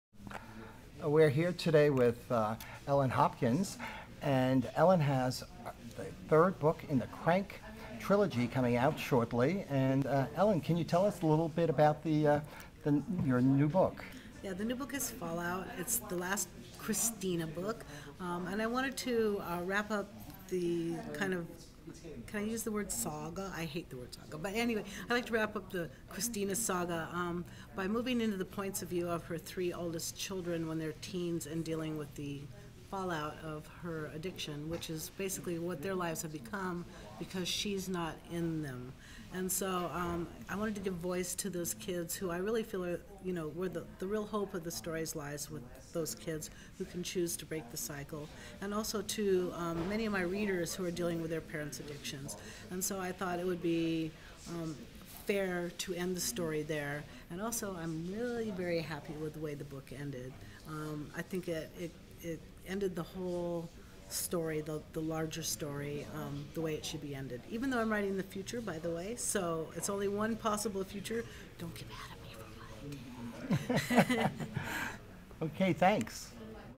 Ellen Hopkins
School Library Journal asks young adult author about her third book in the Crank Triology